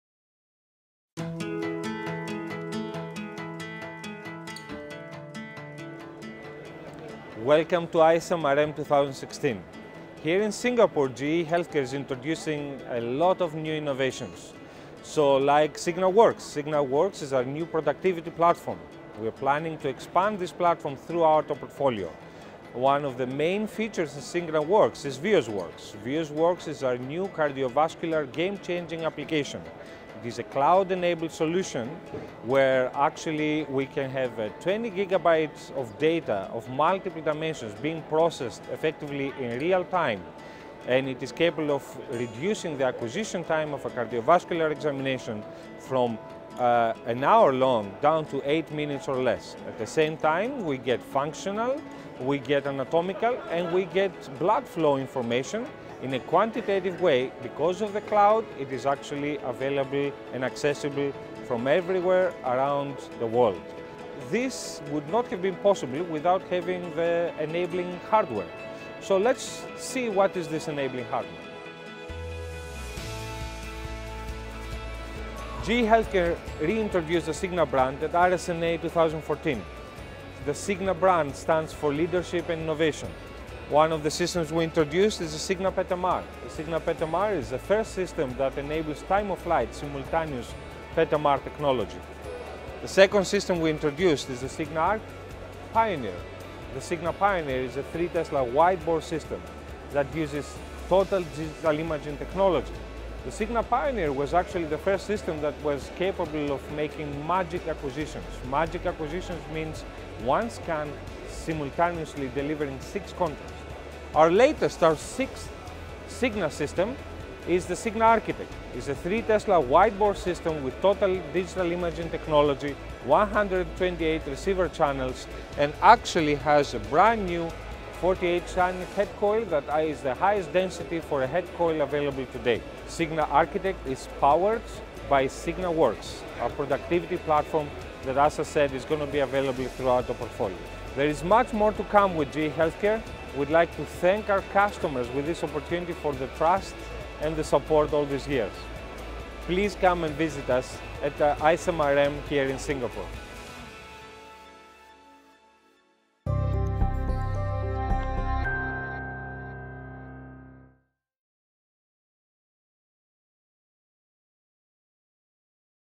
GE Healthcare: Corporate Gold Sponsor - ISMRM 2016
Ioannis Panagiotelis of GE Healthcare leads ISMRM TV through some of the latest technology at their ISMRM 2016 booth.